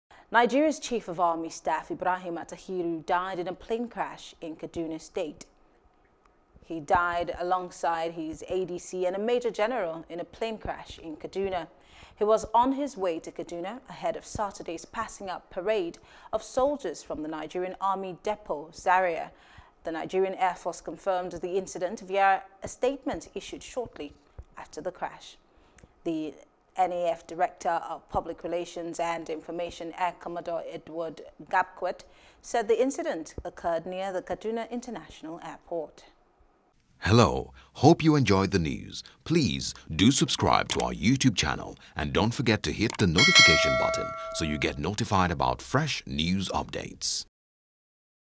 Chief Of Army Staff, Attahiru Dies In Military Plane Crash | NEWS
Nigeria’s Chief of Army Staff Ibrahim Attahiru has died in a plane crash in Kaduna state.